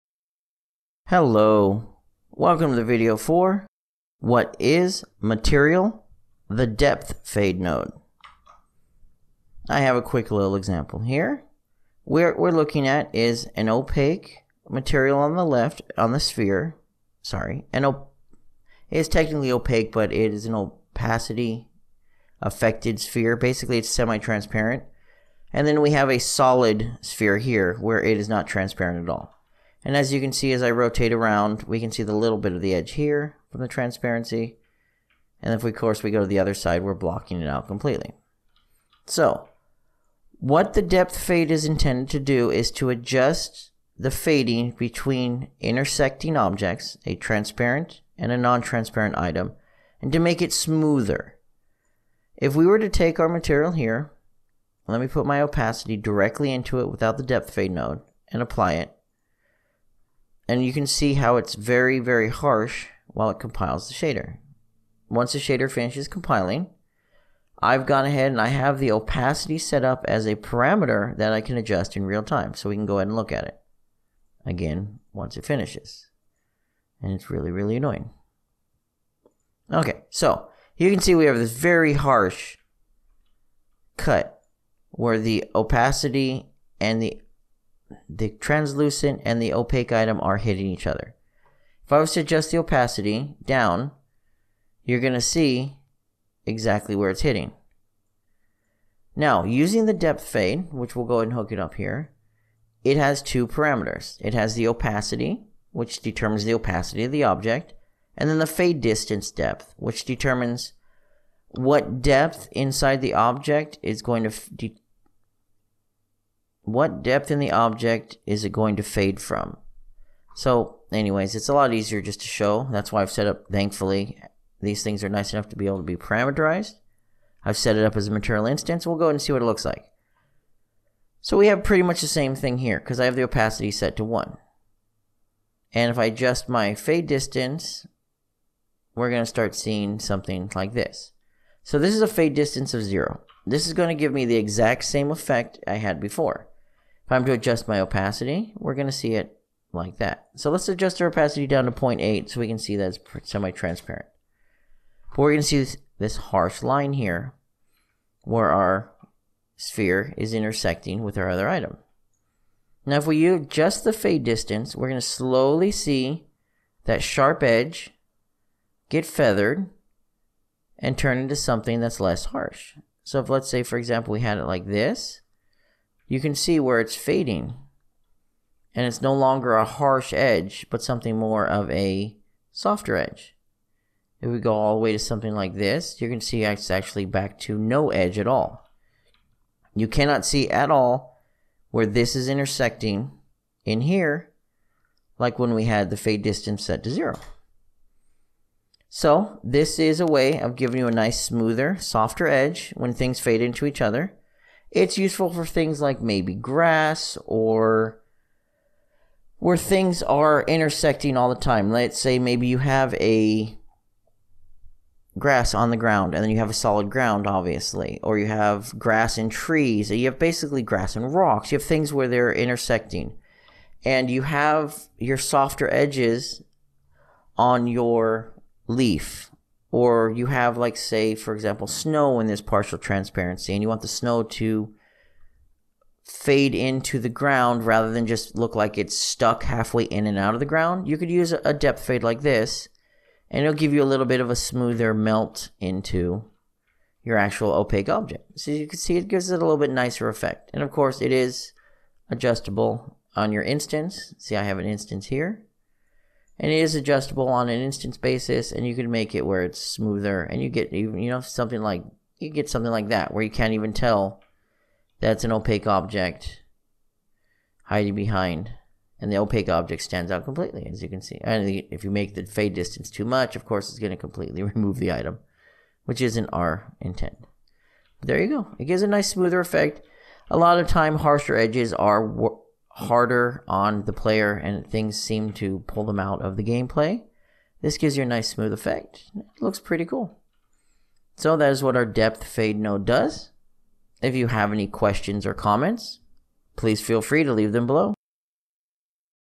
WTF Is? Material - Depth Fade in Unreal Engine 4
What is the Material: Depth Fade Node in Unreal Engine 4